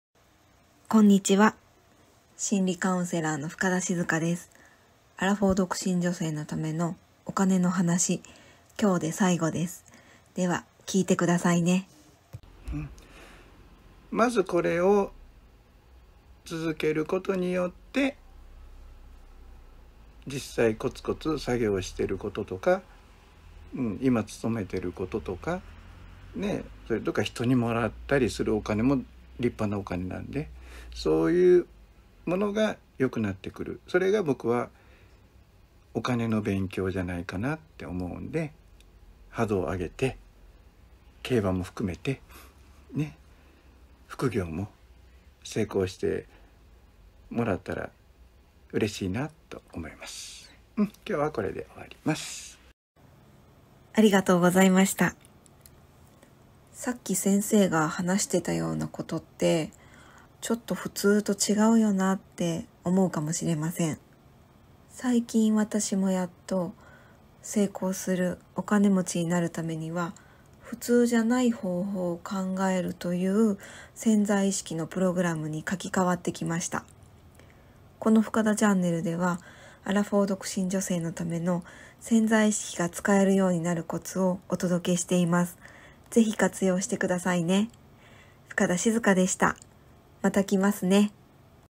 アラ40独身女性の為に必要なお金の話Part16
【今日のおすすめブログ記事】
『無意識・潜在意識のプログラムと書き換え作業の方法を教わった』
ここには普通じゃない考え方が書いていて目からうろこです。
是非読んで見てくださいね。

アラフォー独身女性のための、潜在意識が使えるようになるためのコツをお届けしています。